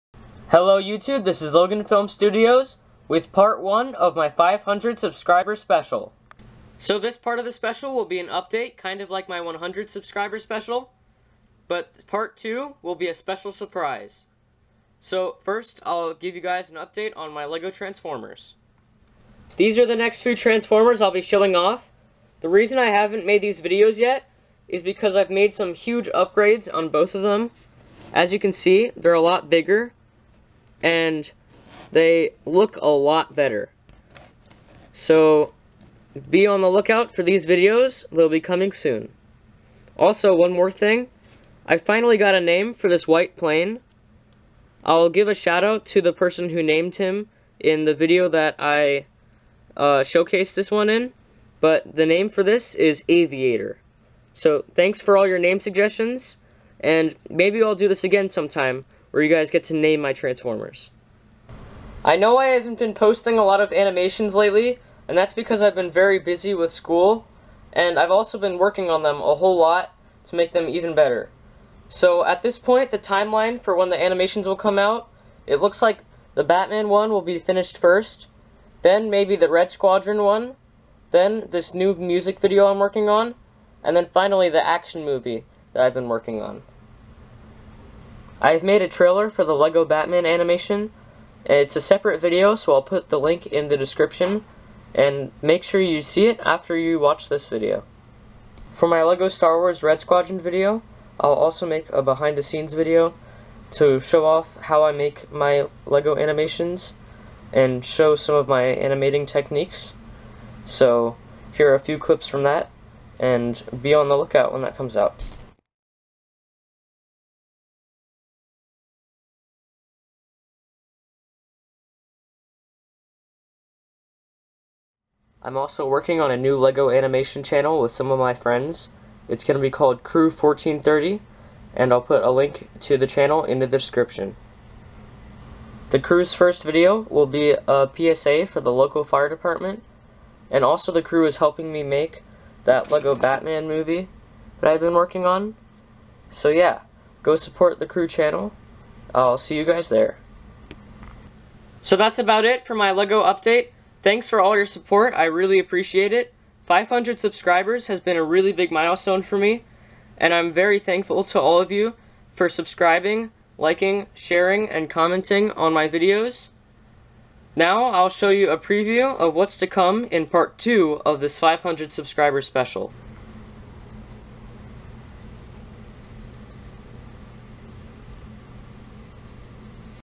Loganfilmstudios LATE 500 Sub Special
Hey everyone!  Sorry this is so late, I've been really busy lately.  Thanks for all you support!  I will try to upload videos more often for now, as I have been making a lot of progress on my LEGO animations and builds.  This is part 1 of the special, where I give you guys a quick update on everything I've been working on.  Part 2 is coming soon and will feature a special surprise!